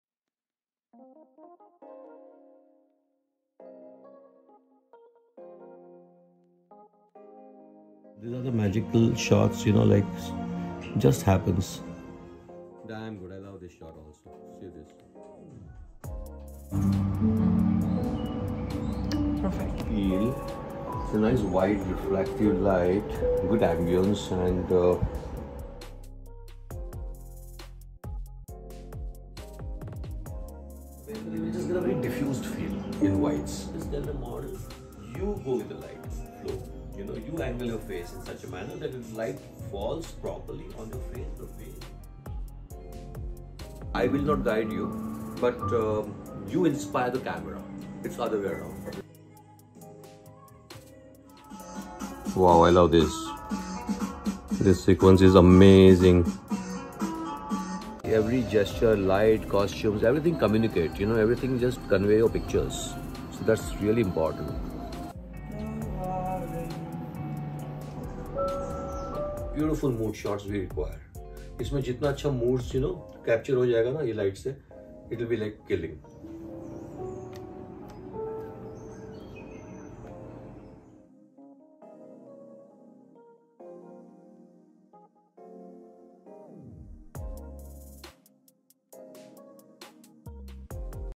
BEHIND THE SCENES | Photography
Behind the scenes video of Jayesh Sheth capturing the beauty of Spriha Sharma.
Shot by Spriha Sharma. 🎥